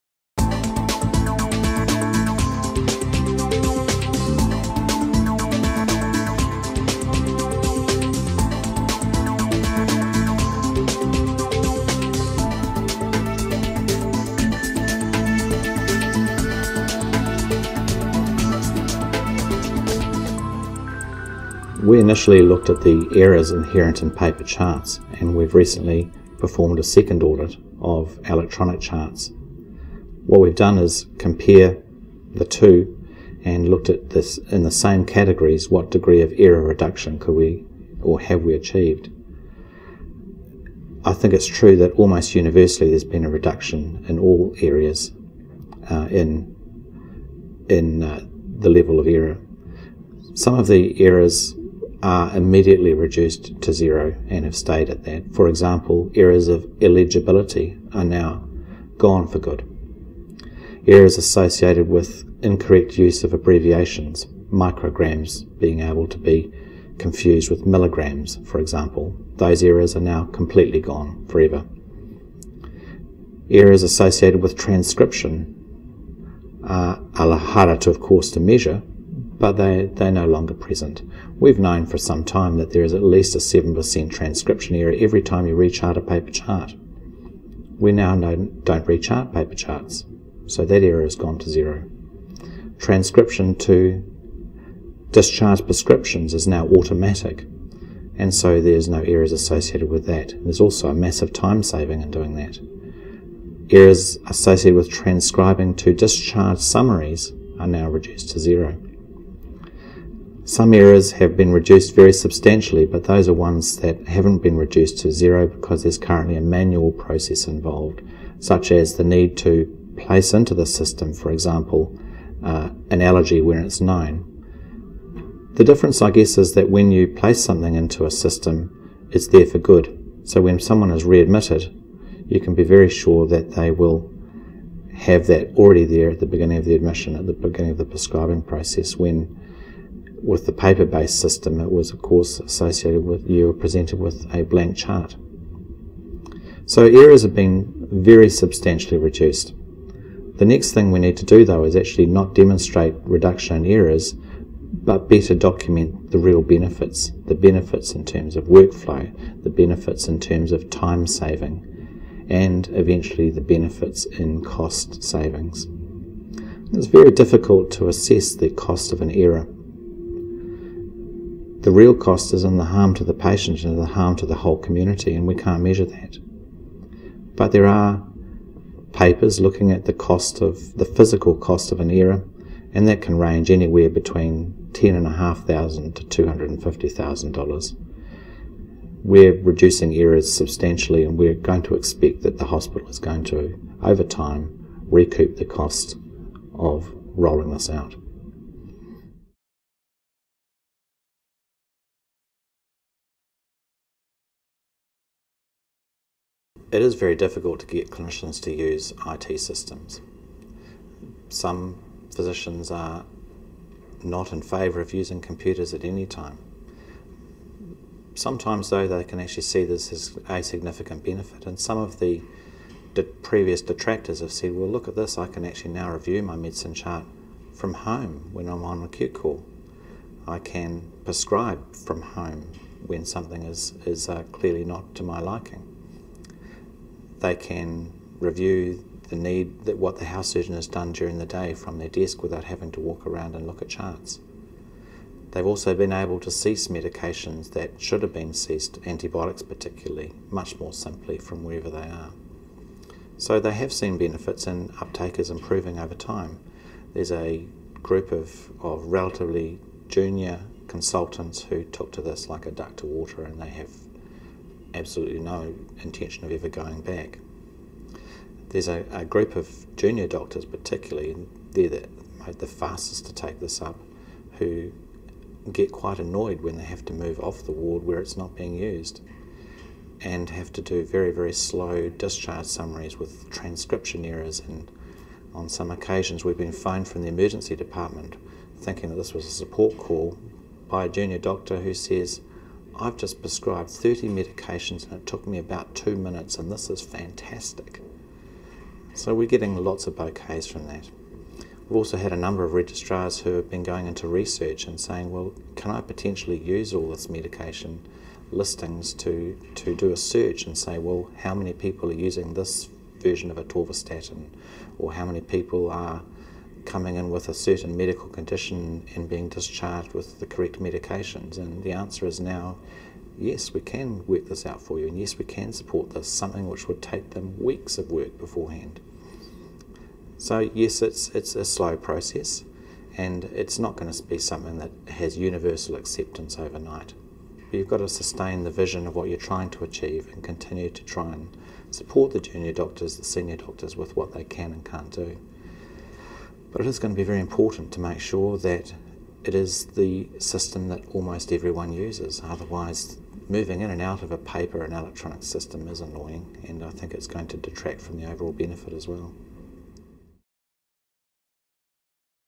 Significant Error Reduction & Clinician Benefits and Uptake | Dr Andrew Bowers
Dr Andrew Bowers, Medical Director of IT, Southern DHB talks about the recent implementation of CSC Medication Management (MedChart) at Dunedin Hospital. Dr Bowers articulates the results of a recent audit comparing errors associated with paper charts verses electronic medication charts. Errors have been reduced substantially and time saving benefits realised, along with reduced manual workflow requirements, such a transcription to discharge prescriptions.